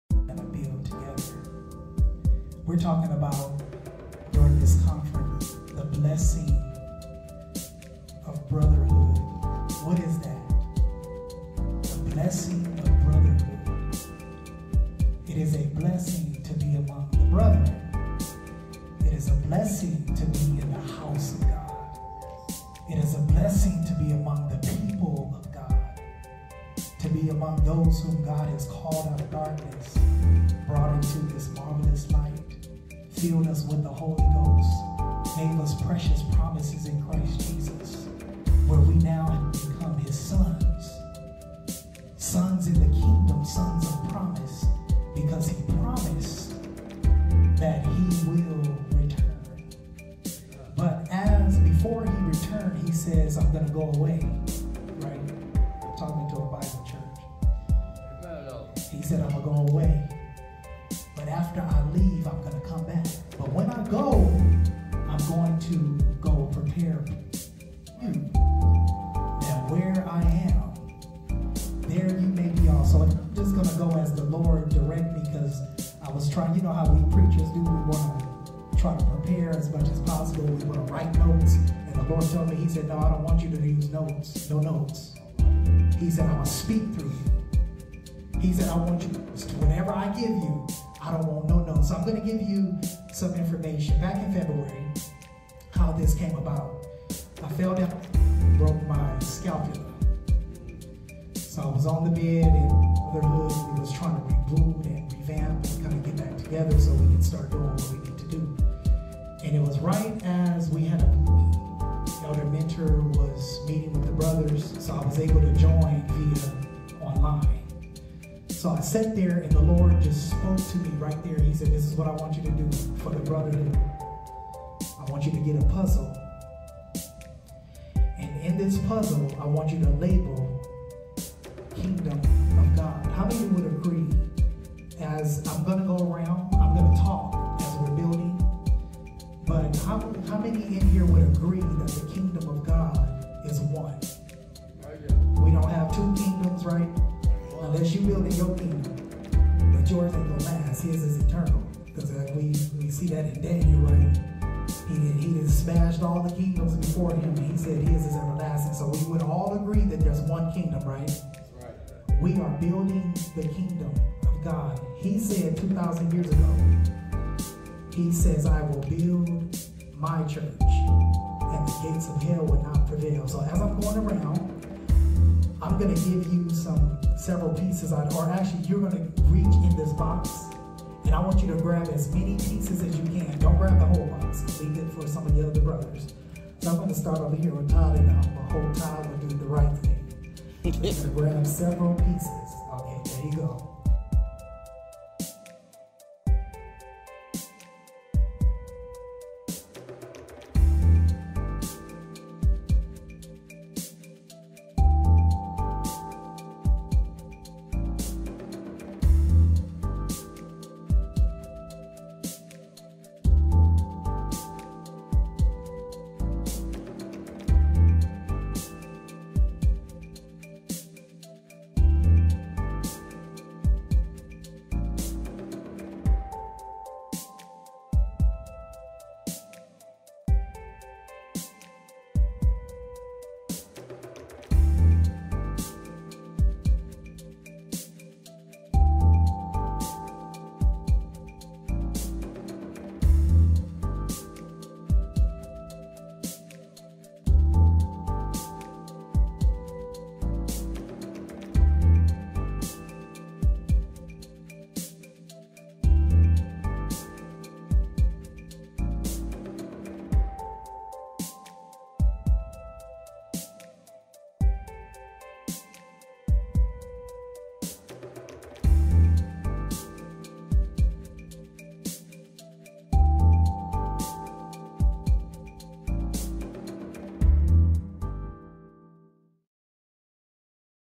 FPC MEN'S MINISTRY SEPTEMBER 16TH 2023
FPC MEN'S MINISTRY CONFERENCE

TITLE: THE BLESSING OF BROTHERHOOD
Elder Romell Alexander

TOPIC: BROTHERHOOD UNITY PUZZLE